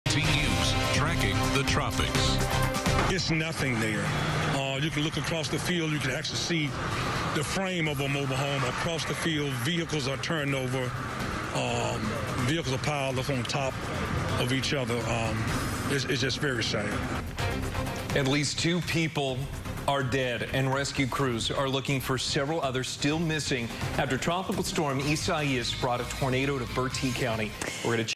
At least 2 dead, 2 missing following Bertie County tornado
At least two people are dead and two are still missing following a tornado in Windsor on Tuesday morning.